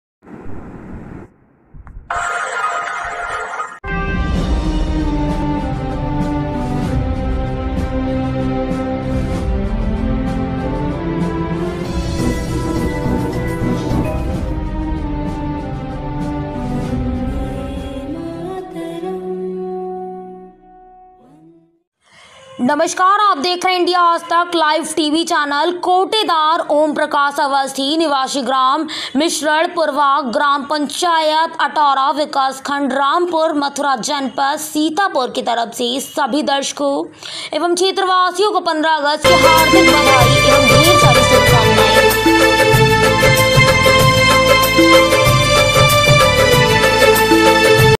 India Aajtak tv live कोटेदार ओम प्रकाश अवस्थी निवासी ग्राम मिश्रण पुरवा ग्राम पंचायत आटौर
shere karo like karo कोटेदार ओम प्रकाश अवस्थी निवासी ग्राम मिश्रण पुरवा ग्राम पंचायत आटौरा विकासखंड रामपुर मथुरा जनपद सीतापुर की तरफ से सभी दर्शकों एवं क्षेत्रवासियों को 15 अगस्त की हार्दिक बधाई एवं ढेर सारी शुभकामनाएं